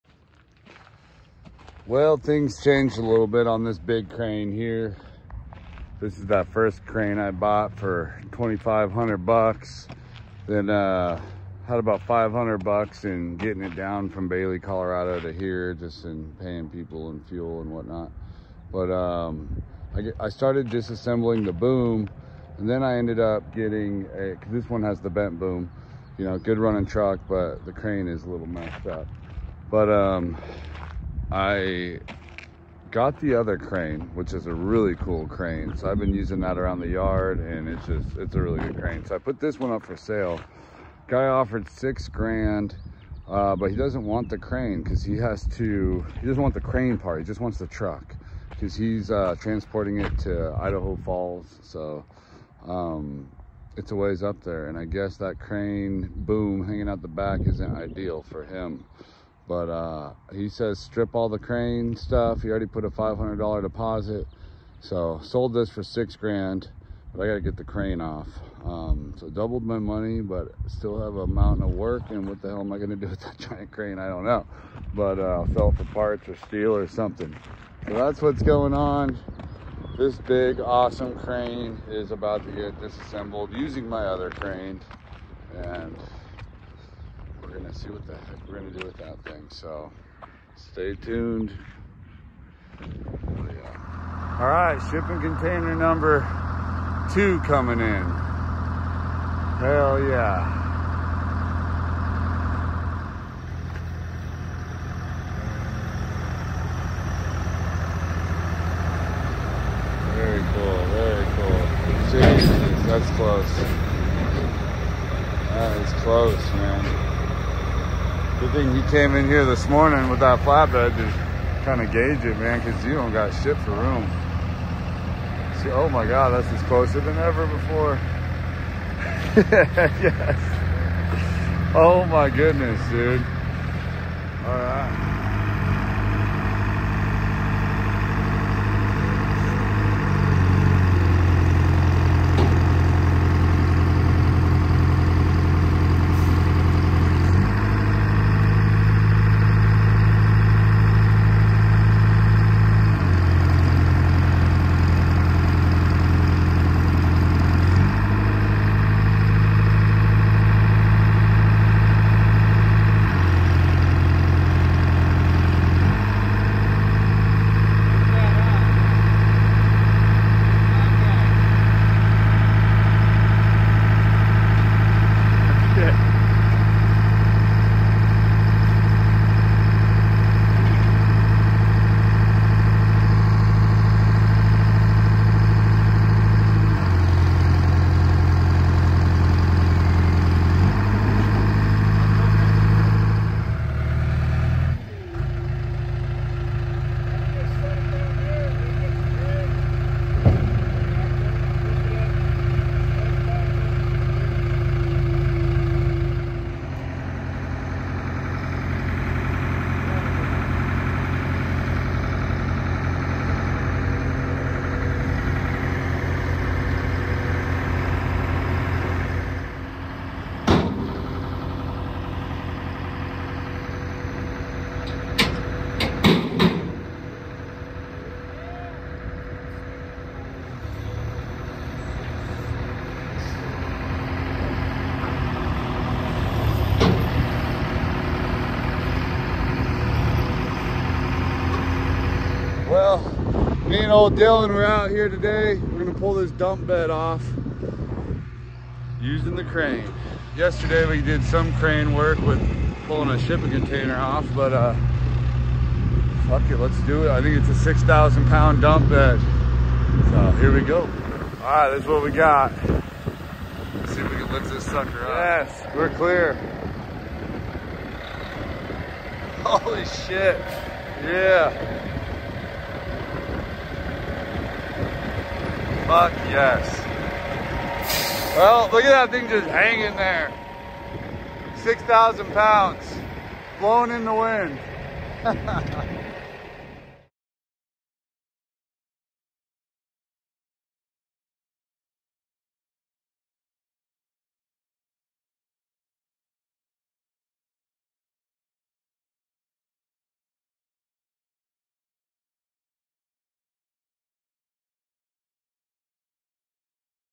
Crane on the Homestead!
Finally get to use a crane! Second shipping container gets to the Homestead. Dump truck bed removal with Crane. ￼Boom truck update.